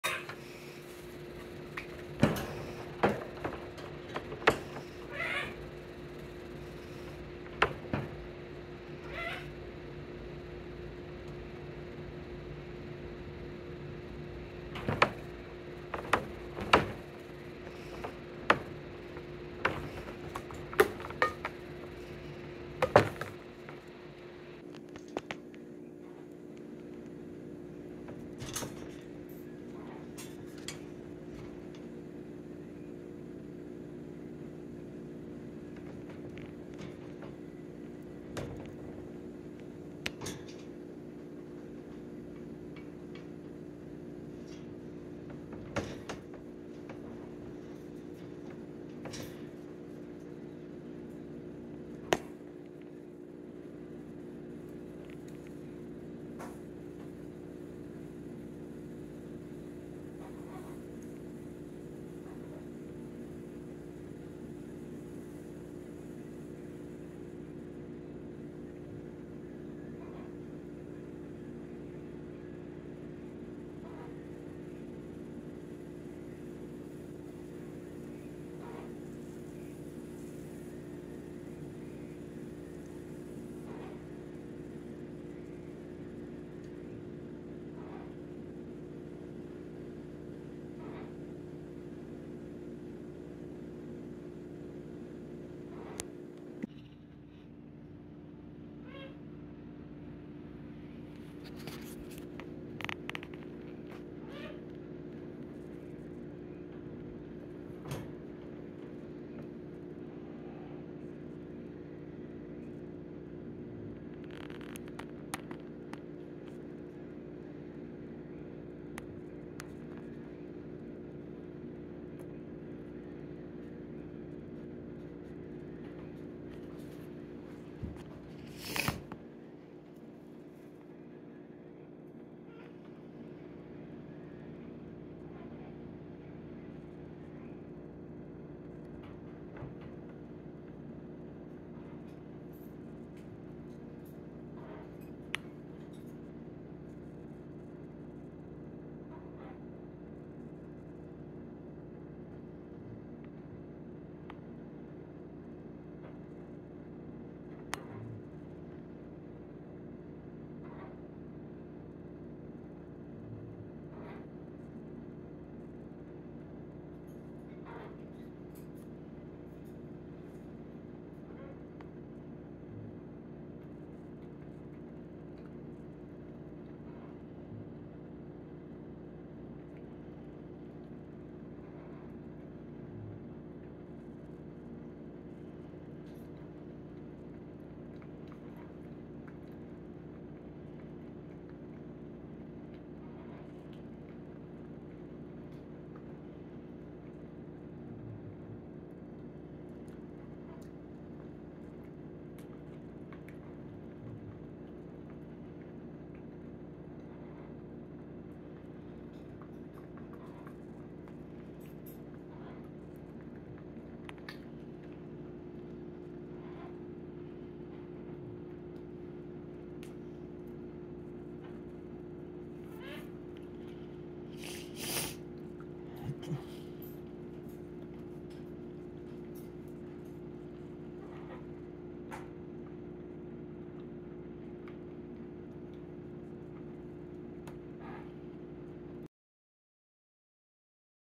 Little Dove progress. Senior deaf toy poodle rescued from hoarder. available for adoption Raleigh,NC
Little Dove is available for adoption in Raleigh, North Carolina through Marden's Ark Dog Tales. She is deaf and possible blindness in one. She needs some TLC but has the potential to be a lovely companion. She is actually very much a little pig, but too shy to eat in front of me until I took this video. It took her 48 mins to finally relax enough to eat in front of my presence🥺